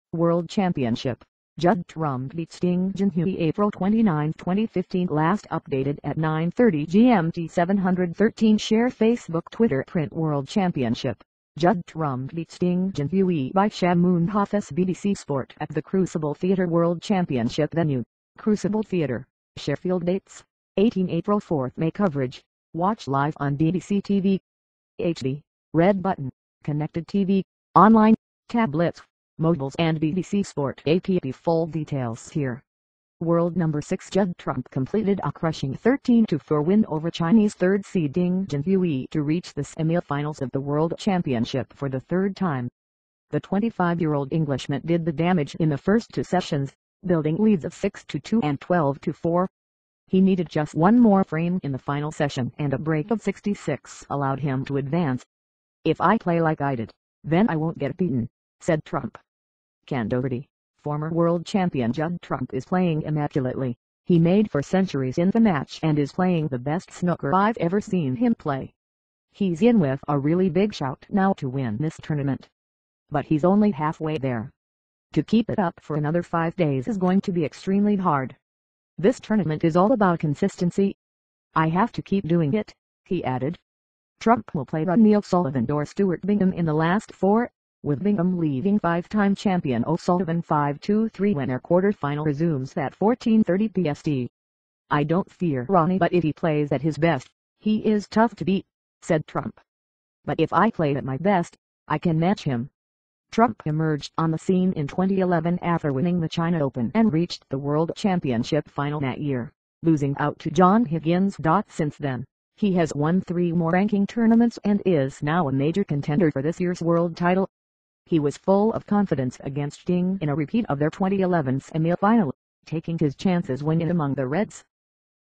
World Championship: Judd Trump beats Ding Junhui
29 April 2015
Last updated at 09:30 GMT
713

By Shamoon Hafez
BBC Sport at the Crucible Theatre
World Championship
Venue: Crucible Theatre, Sheffield Dates: 18 April-4 May
Coverage: Watch live on BBC TV, HD, Red Button, Connected TV, online, tablets, mobiles and BBC Sport app. Full details here.
World number six Judd Trump completed a crushing 13-4 win over Chinese third seed Ding Junhui to reach the semi-finals of the World Championship for the third time.
The 25-year-old Englishman did the damage in the first two sessions, building leads of 6-2 and 12-4.
He needed just one more frame in the final session and a break of 66 allowed him to advance.
"If I play like I did, then I won't get beaten," said Trump.
Ken Doherty, former world champion
"Judd Trump is playing immaculately - he made four centuries in the match and is playing the best snooker I've ever seen him play. He's in with a really big shout now to win this tournament. But he's only halfway there."
"To keep that up for another five days is going to be extremely hard. This tournament is all about consistency. I have to keep doing it," he added.
Trump will play Ronnie O'Sullivan or Stuart Bingham in the last four, with Bingham leading five-time champion O'Sullivan 5-3 when their quarter-final resumes at 14:30 BST.
"I don't fear Ronnie but if he plays at his best, he is tough to beat," said Trump. "But if I play at my best, I can match him."
Trump emerged on the scene in 2011 after winning the China Open and reached the World Championship final that year, losing out to John Higgins.Since then, he has won three more ranking tournaments and is now a major contender for this year's world title.
He was full of confidence against Ding in a repeat of their 2011 semi-final, taking his chances when in among the reds